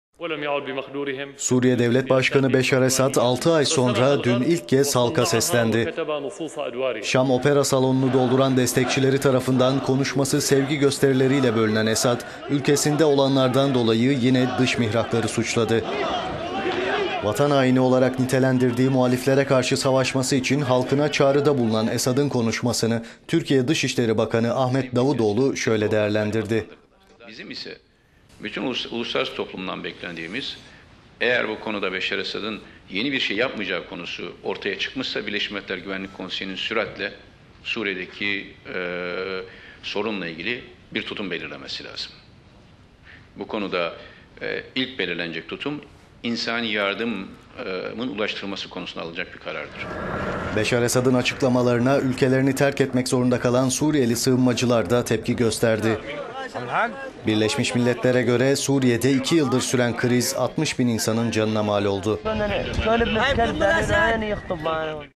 Davutoğlu'ndan BMGK'ya Suriye çağrısı
Suriye Devlet Başkanı Beşar Esad, altı ay sonra dün ilk kez halka seslendi. Şam Opera Salonu'nu dolduran destekçileri tarafından konuşması sevgi gösterileriyle bölünen Esad, ülkesinde olanlardan dolayı yine dış mihrakları suçladı.

'Vatan haini' olarak nitelendirdiği muhaliflere karşı savaşması için halkına çağrıda bulunan Esad'ın konuşmasını Türkiye Dışişleri Bakanı Ahmed Davutoğlu şöyle değerlendirdi: "Eğer bu konuda Beşar Esad'ın yeni birşey yapmayacağı konusu ortaya çıkmışsa Birleşmiş Milletler Güvenlik Konseyi'nin süratle Suriye'deki sorunla ilgili bir tutum belirlemesi lazım. Bu konuda ilk belirlenecek tutum, insani yardımın ulaştırılması konusunda alınacak bir karardır."

Beşar Esad'ın dünkü konuşmasına, ülkelerini terketmek zorunda kalan Suriyeli sığınmacılarda da tepki gösterdi. Amerika Birleşik Devletleri, beş aşamalı bir barış planı sunan, ancak Suriye dışındaki muhaliflere diyalog kapısını kapatan Esad'ı kınadı.

Birleşmiş Milletler'e göre Suriye'de iki yıldır süren kriz 60 bin insanın canına mal olmuş durumda.